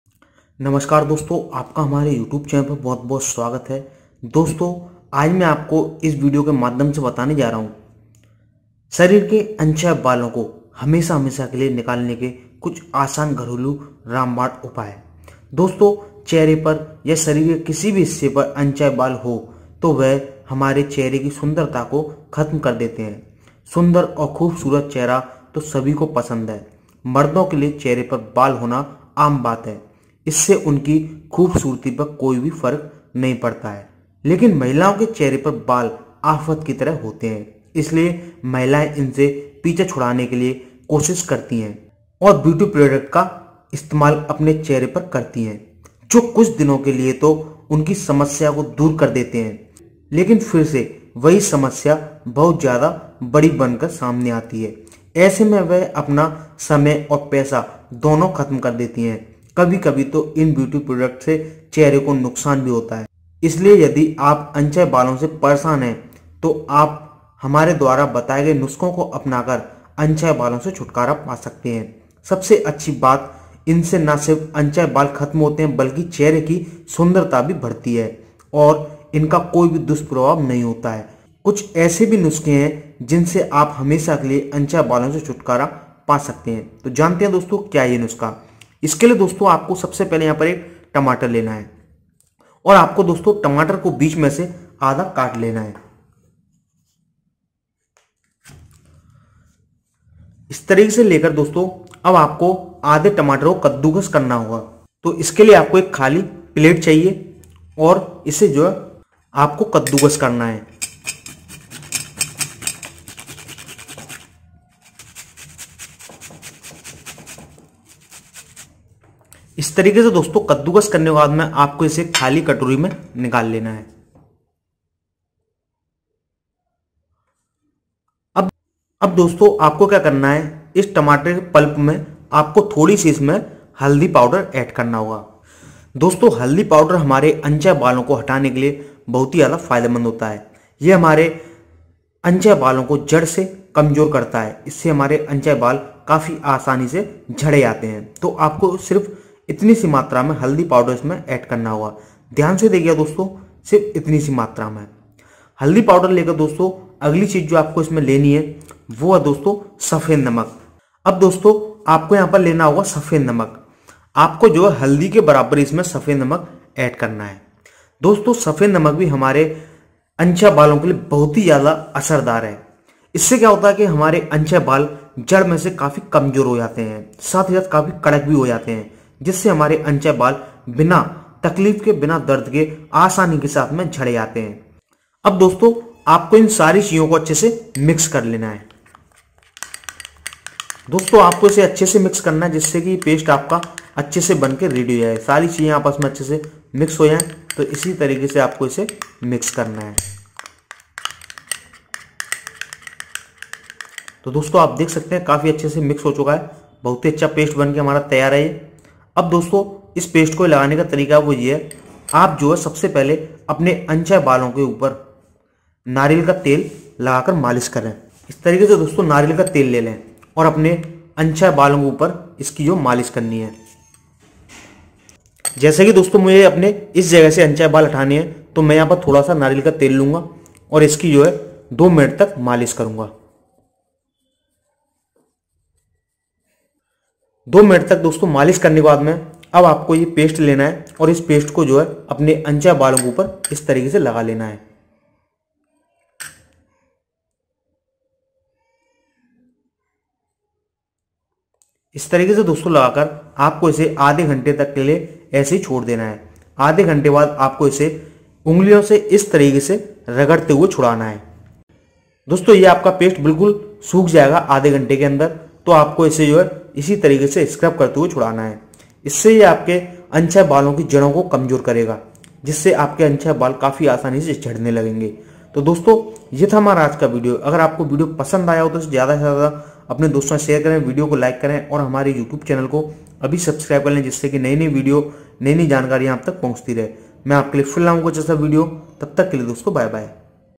2 रूपये की ये चीज लगाई शरीर के अनचाहे बाल हमेशा के लिए ऐसे झड़ जायेंगे जैसे कभी थे ही नहीं!
2 रूपये की ये चीज लगाई शरीर के अनचाहे बाल हमेशा के लिए ऐसे झड़ जायेंगे जैसे कभी थे ही नहीं!